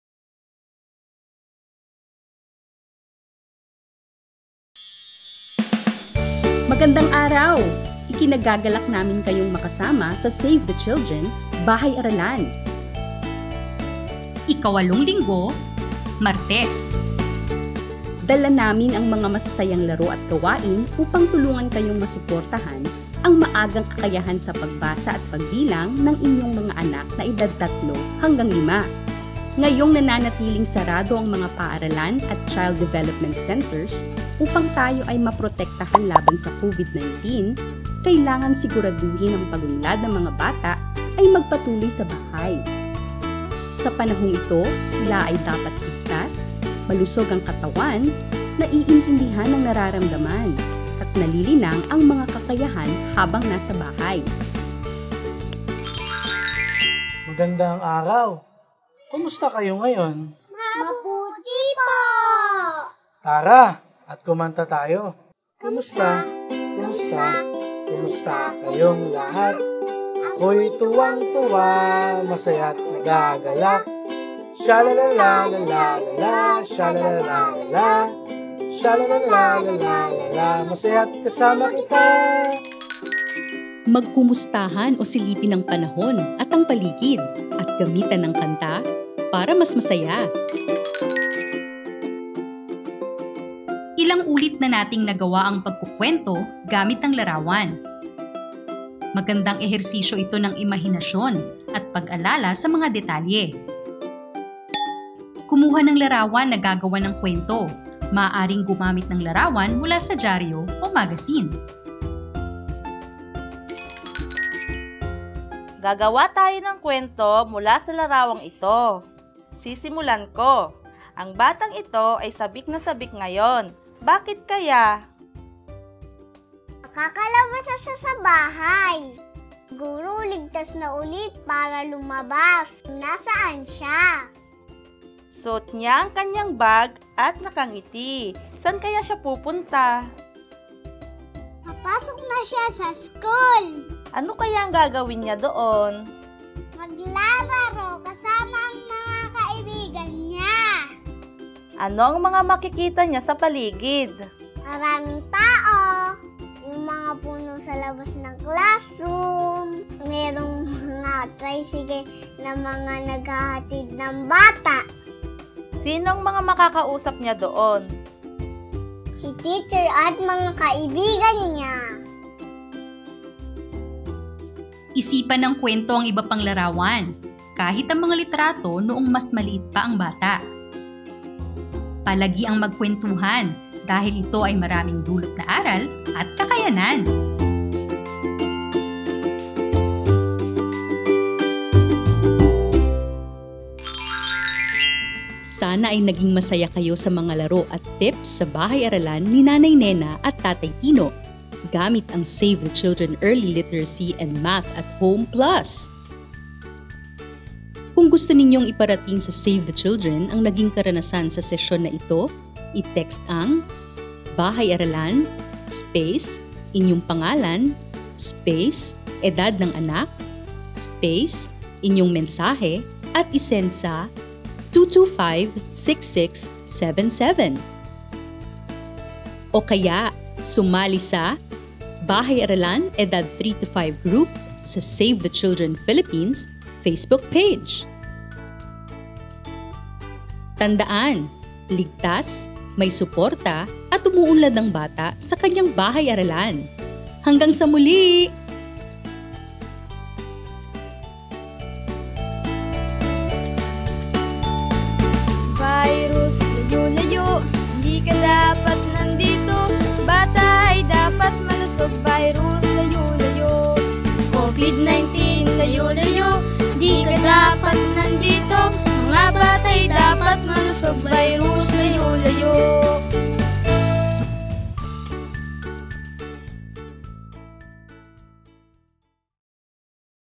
BahayAralan: Edad 3-5 (Linggo 8, Martes)
Ika-walong linggo na ng ating BahayAralan. Sa ating mga nakaraang gawain, ilang ulit na natin nagawa ang pagkukwento gamit ang larawan. Magandang ehersisyo ito ng imahinasyon at pag-alala sa mga detalye. Handa na ba kayo gumawa ng kwento gamit ang mga larawan?

Ang BahayAralan para sa mga batang edad 3-5 ay mga maiikling sesyon ng masasayang laro at gawain na tatagal ng 12 linggo, gamit ang aming Early Literacy and Math (ELM) at Home Plus+ modules. Mapapanuod ito mula Lunes hanggang Biyernes sa Save the Children Philippines Facebook